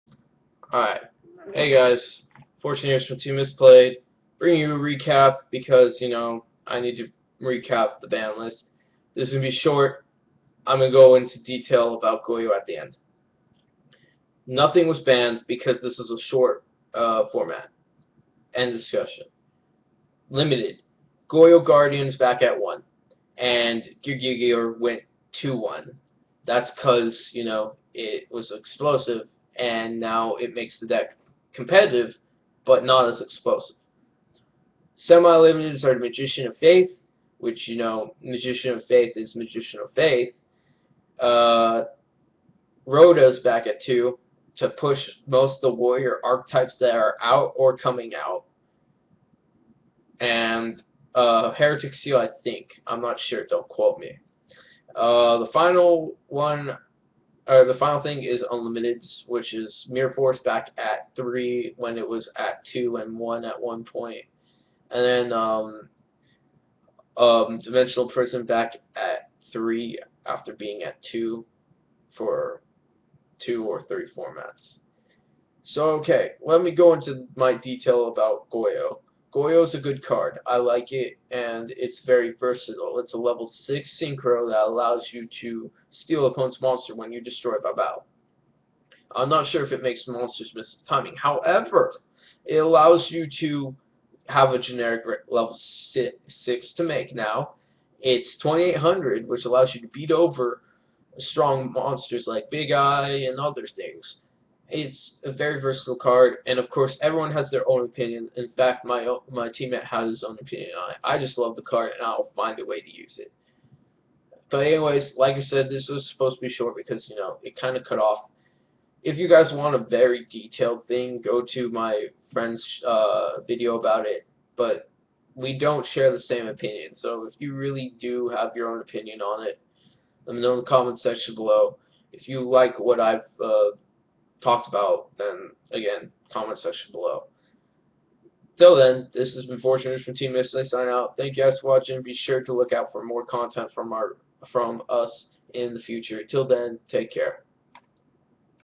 FortuneHeros Quick Recap Banlist 7/15/14
FortuneHeros recap on banlist and why he likes Goyo

Also I realize that the card wasn't Heratic Seal, it was Formula Synchron